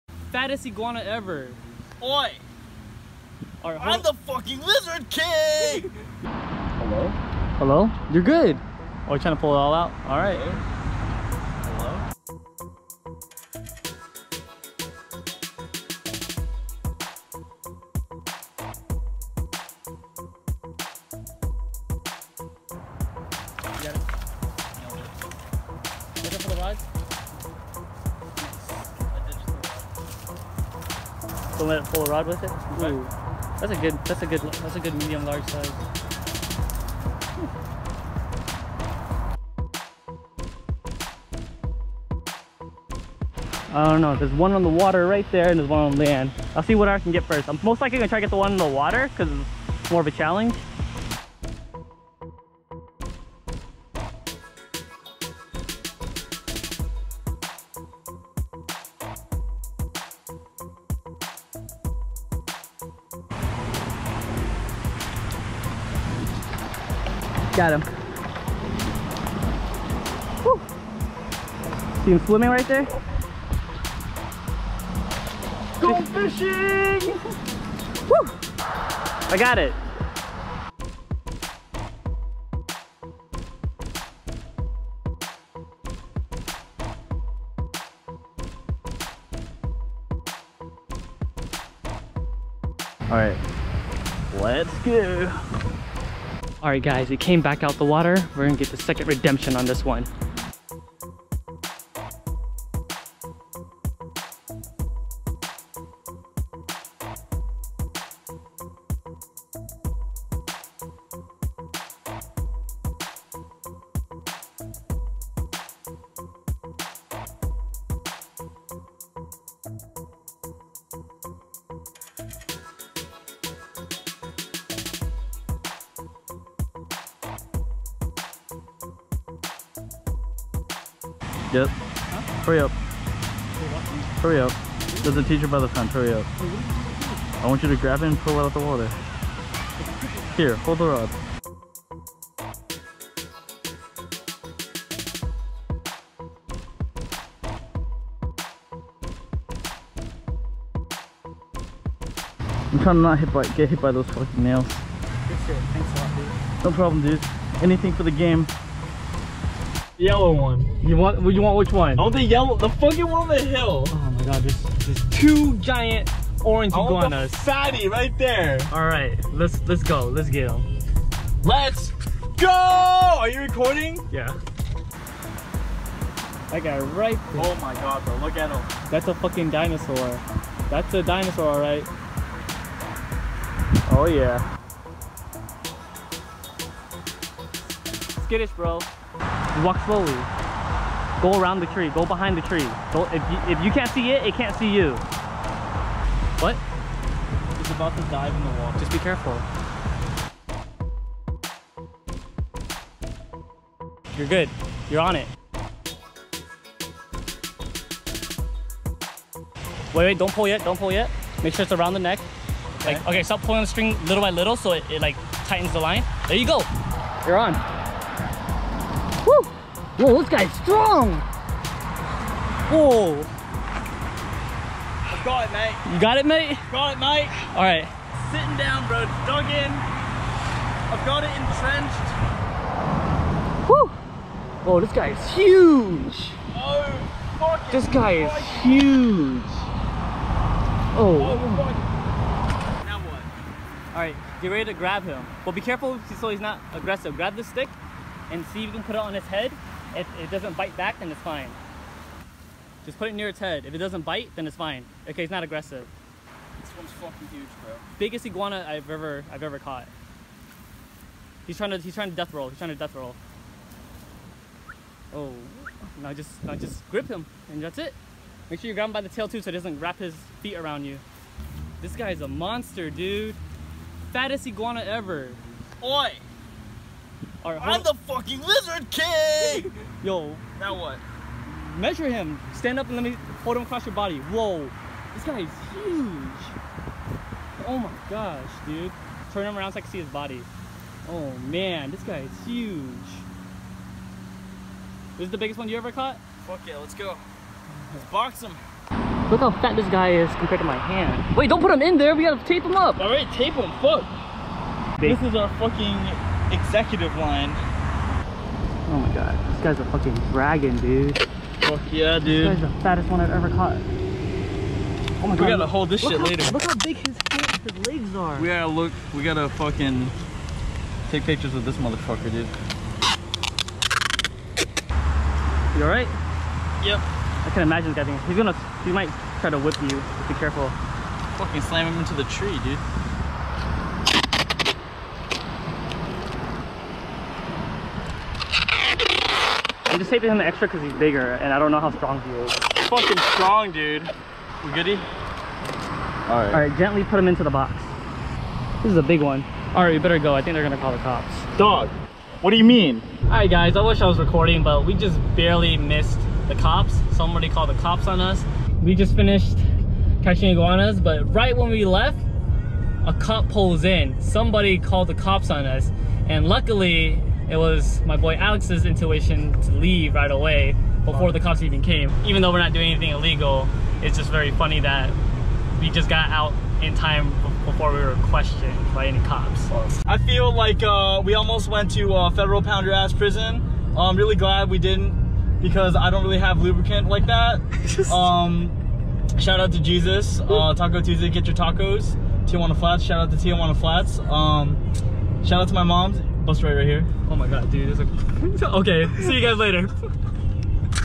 Biggest/Fattest Iguana Ever Caught! | Iguana Time
Me and the Lizard King hunting Giants out here. Lmk if you would love more videos with the lizard king.